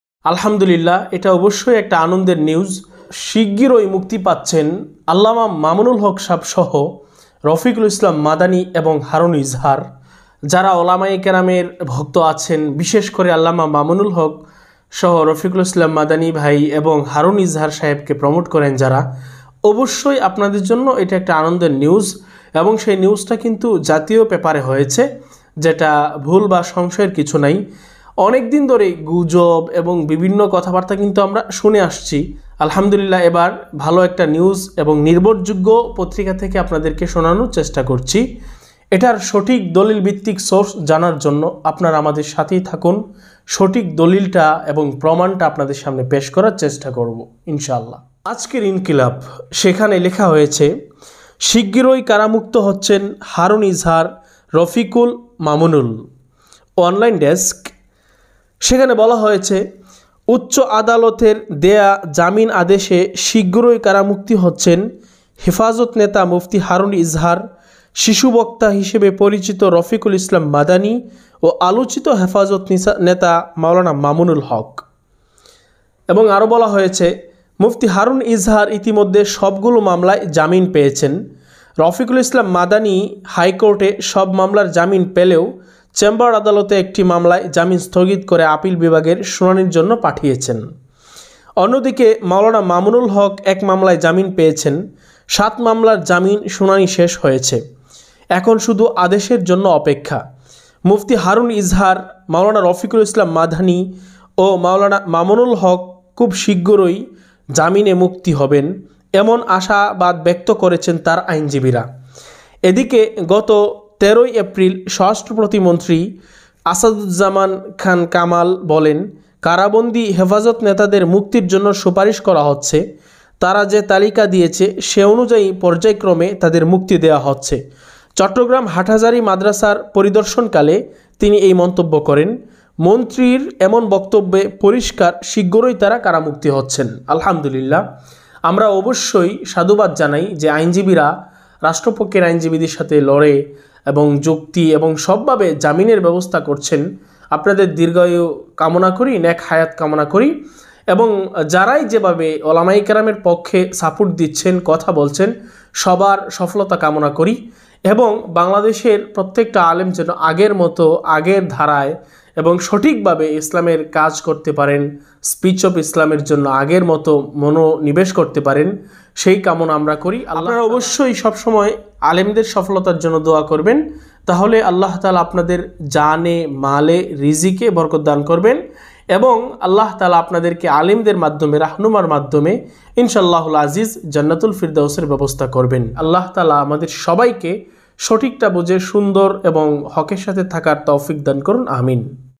আলহামদুলিল্লাহ- অবশেষে জামিন পেলেন মামুনুল হক ও শিশু বক্তা রফিকুল ইসলাম মাদানী এবং হারুন ইজহার
খুব শীঘ্রই মুক্তি পাচ্ছে
আল্লামা মামুনুল হক
মুফতি হারুনী ইজহার
শিশু বক্তার রফিকুল ইসলাম মাদানী
Allama Mamunul haque
mufti Harun Ejhar
mawlana rafiqul Islam Madani